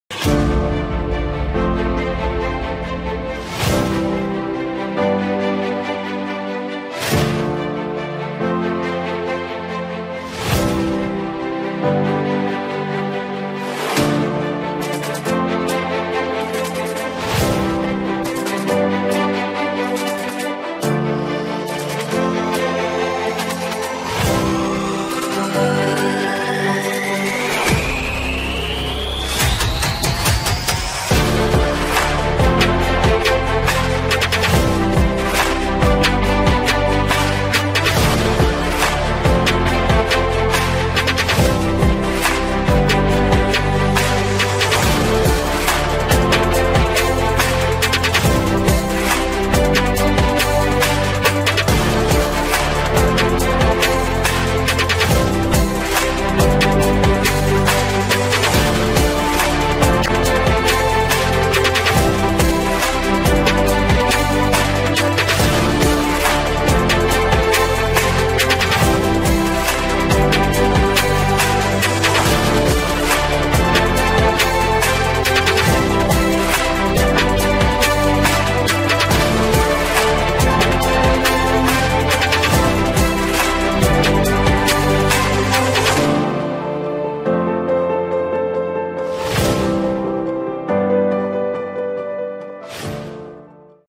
Impulse für den Mittelstand 2022
Programm des Symposiums "Impulse für den Mittelstand 2022" in Baden-Baden, Kurhaus. Veranstaltet von der GfA e.V.